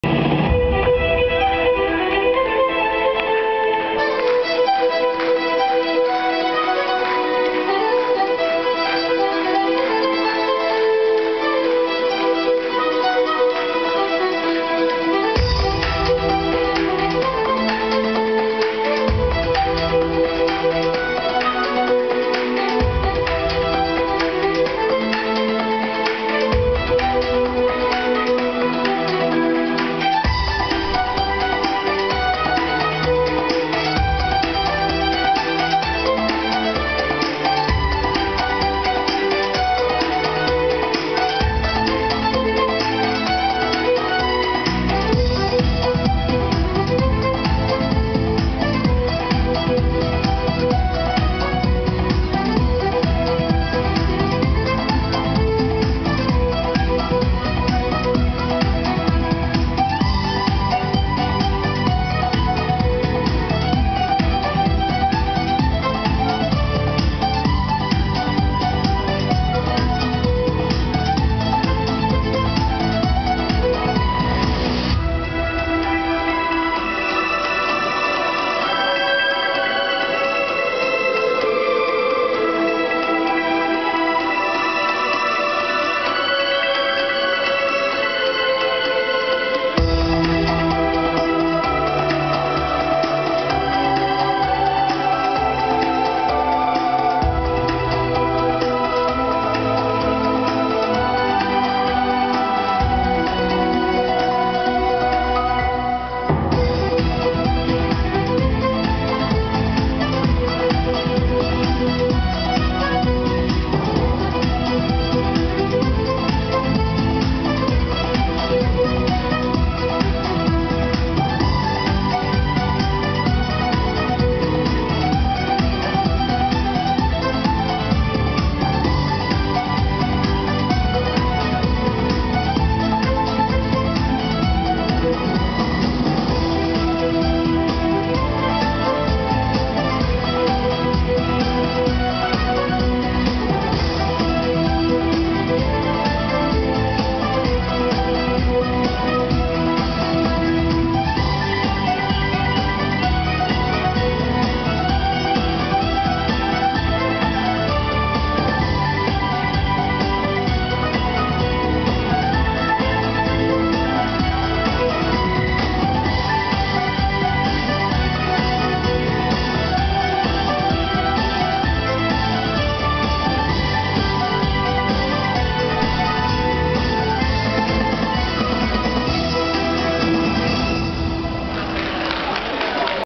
CASTELNAU D ' ESTRETEFONDS (31) - Country Line Dance - S 14 Mai 2011 (V04)
(Vidéo légerement trouble, cause mauvais réglage : Désolé ..sniffff )

Agréable et conviviale Soirée Line dance Country , à CASTELNAU D' ESTRETEFONDS (Hte Garonne) ce samedi 14 Mai 11.